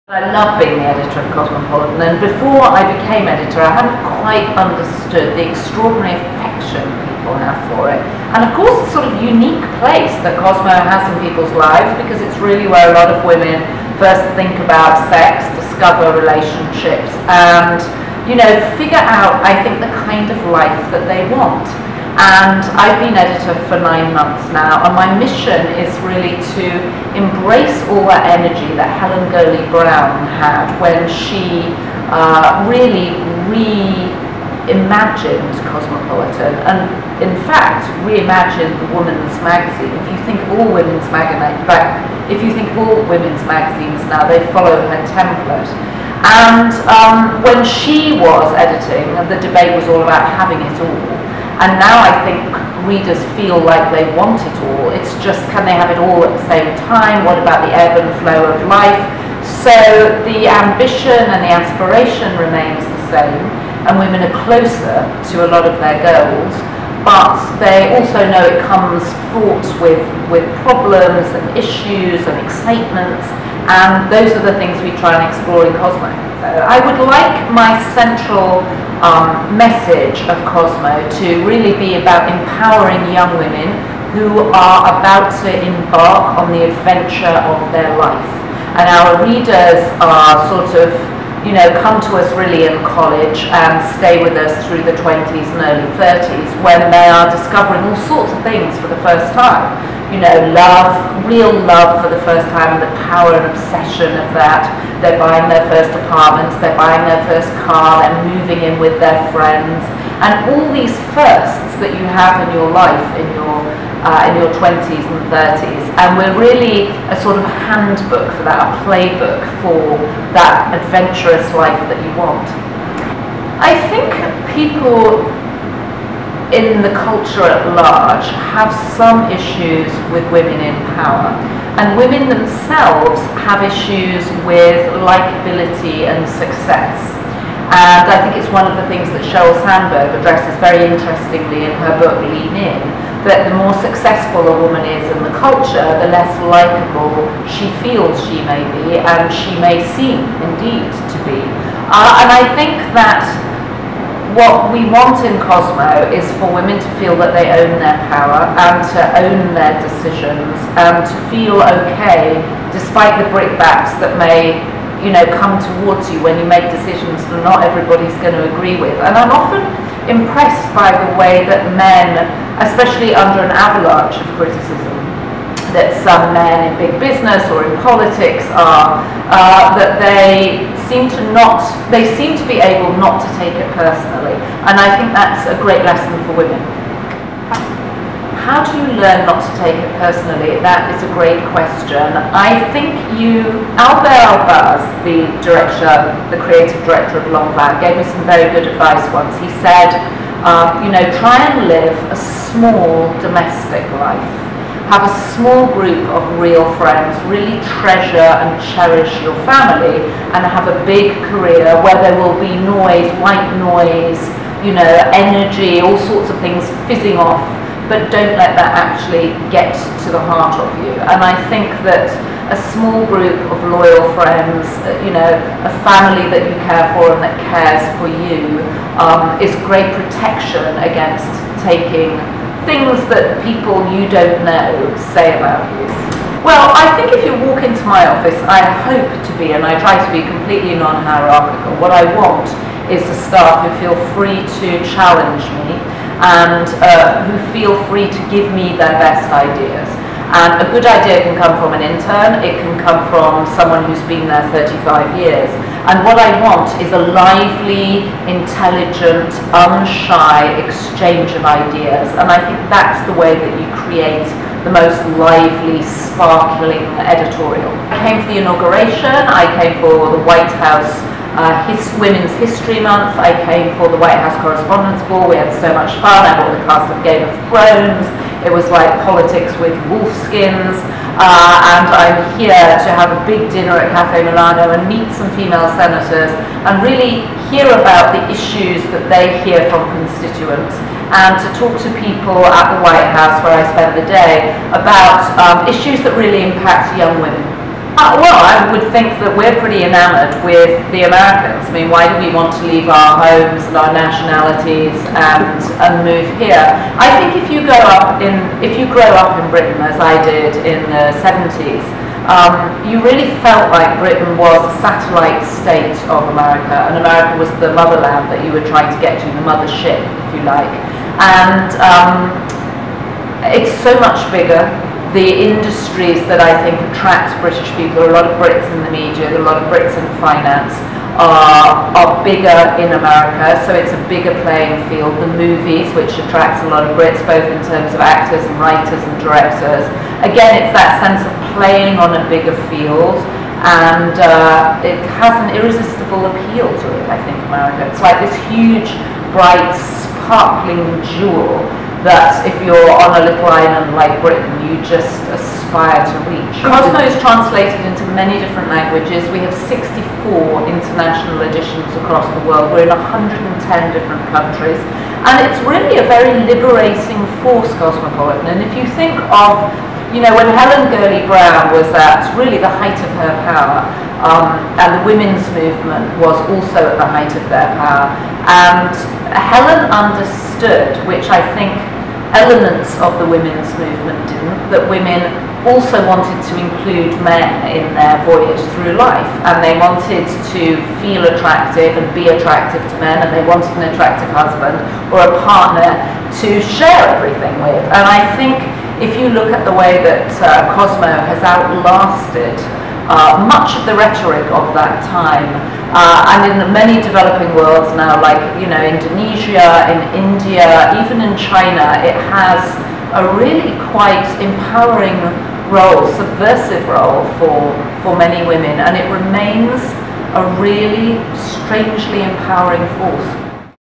Joanna Coles - Editor in Chief - Cosmopolitan Magazine: Hollywood on the Potomac
If hobnobbing is the art of social interaction with 'higher-ups,' then Joanna Coles - new Editor-in-Chief of Cosmopolitan Magazine - does it well.  That's not to say she isn't herself one of the most powerful women in the world, but when Minority Leader Nancy Pelosi and former Secretary of State and Four Star General Colin Powell did a spontaneous drop in at a private dinner in her honor - that we guess, is hobnobbing at its best and exactly what took place at Cafe Milano.